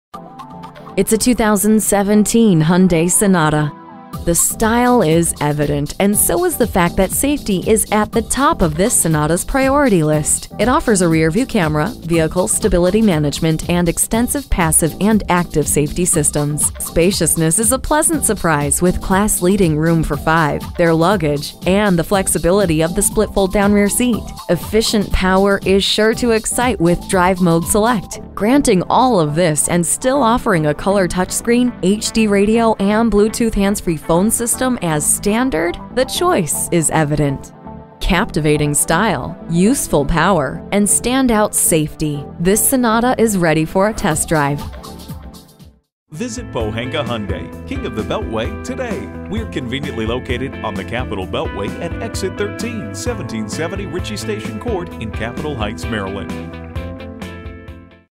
New 2017 Hyundai Sonata Capitol Heights MD Washington-DC, MD FHH454962 - SOLD
Year: 2017
Make: Hyundai
Model: Sonata
Trim: Sport
Engine: 2.4L I4 DGI DOHC 16V PZEV 185hp
Transmission: 6-Speed Automatic with Shiftronic
Color: Brown
Stock #: FHH454962
VIN: 5NPE34AF5HH454962
 Price includes: $1,750 - Retail Bonus Cash - MY17 Sonata - CE/WE/SC/SO. Exp. 02/28/2017

Address:
1770 Ritchie Station Court